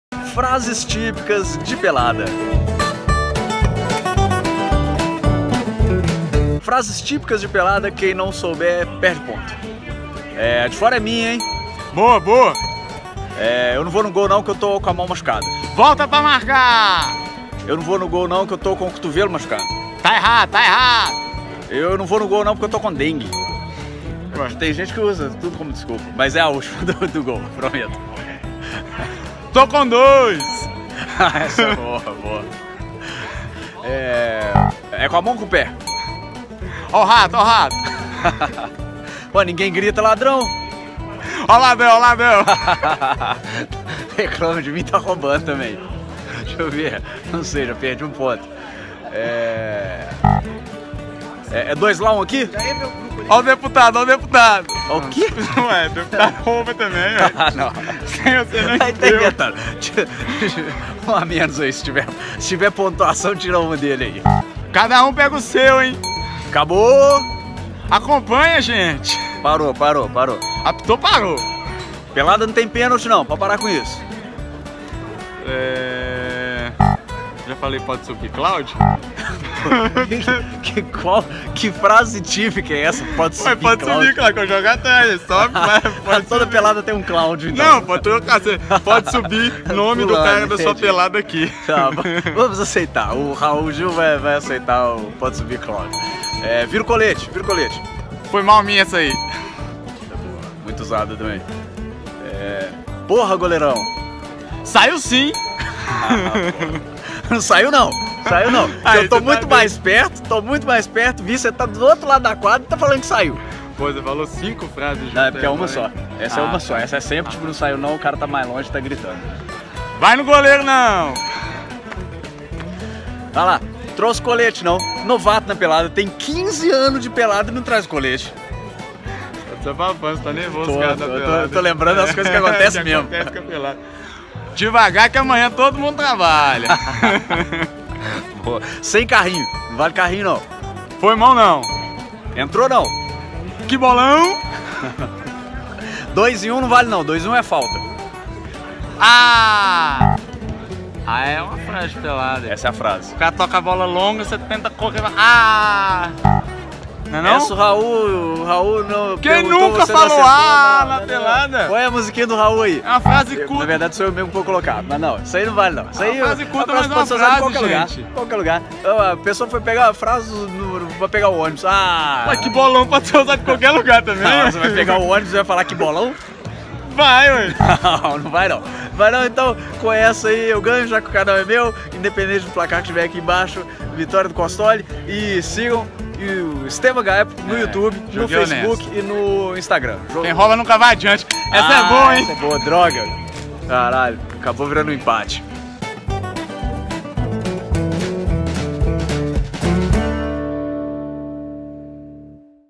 COISAS QUE TODO PELADEIRO FALA - BRUNO COSTOLI
Se você joga bola, vai reconhecer vários amigos (ou inimigos) nessas frases!

Tem vários outros vídeos no canal, assiste aí. :D